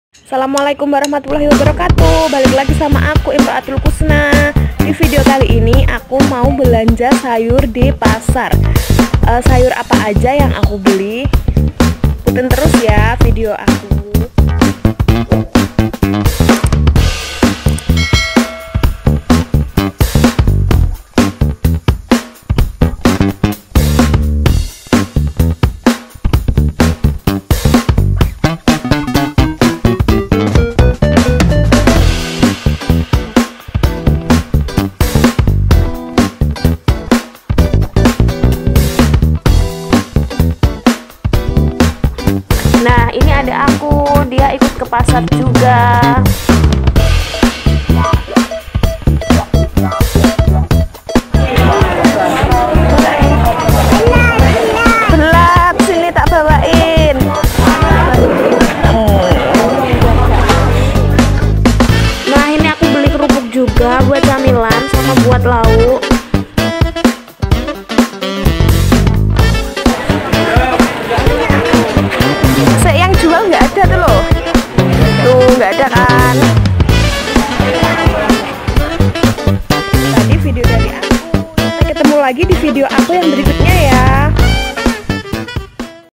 Belanja sayur di pasar !!!
Kali ini aku belanja sayur di pasar tradisional dekat rumah aku
Harga disini dijamin murah
Buat kalian yang gak bakat nawar , bisa belanja disini 😃